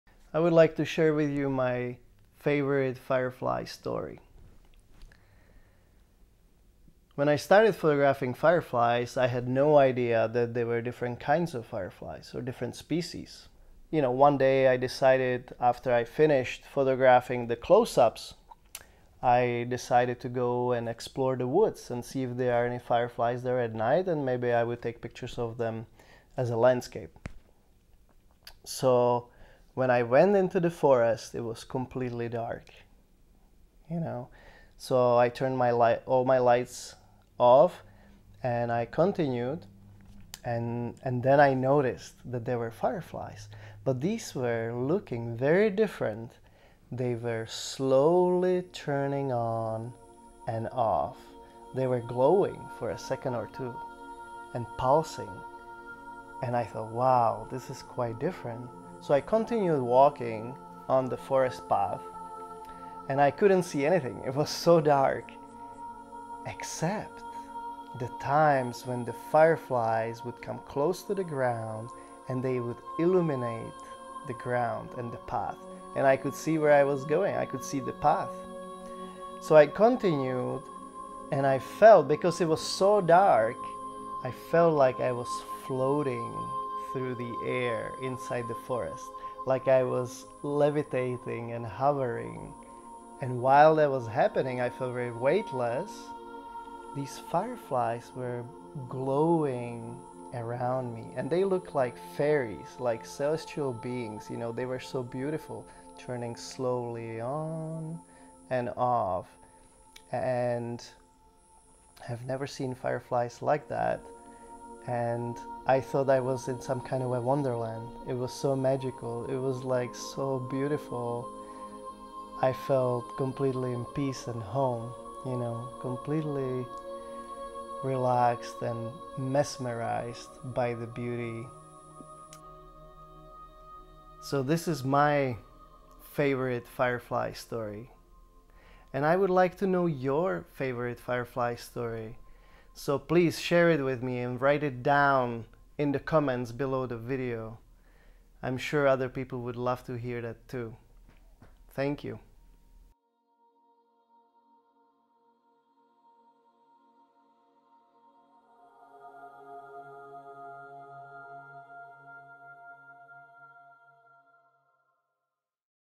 My Favorite Firefly Story
What is your favorite firefly story?

When we were doing art shows we used to hear many fireflies stories from our customers. Some of the stories where so beautiful and touching that we thought it would be great to have a space to share them here every Wednesday. We would like to select the best stories and feature them in our blog and perhaps in the future create a firefly stories book. What is your favorite firefly story?